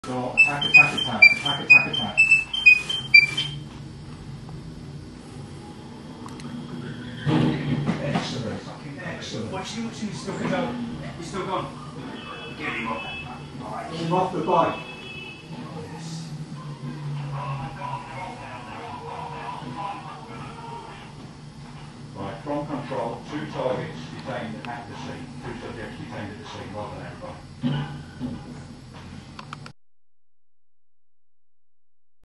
Police RUN OVER Motorcycle Bank robbers in UK, Awesome move, busted arrested
Motorcycle Bandits RUN OVER by Police in UK, Awesome move, busted arrested